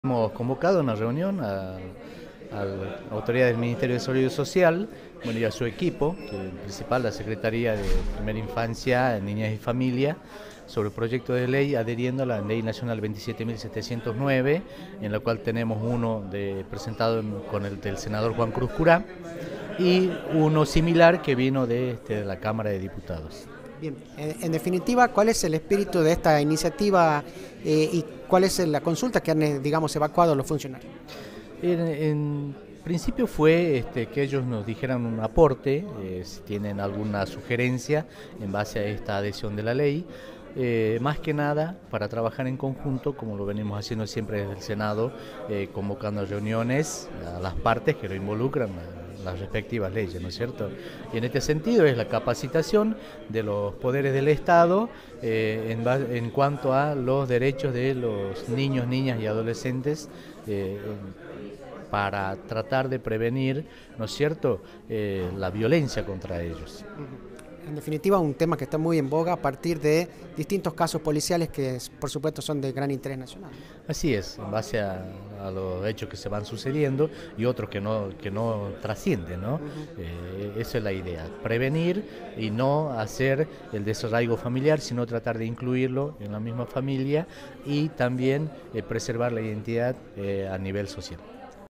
CARLOS AMPUERO - SENADOR PROVINCIAL
ANALIZAN LA ADHESIÓN AL PLAN FEDERAL DE CAPACITACIÓN SOBRE DERECHOS DEL NIÑOS, NIÑAS Y ADOLESCENTES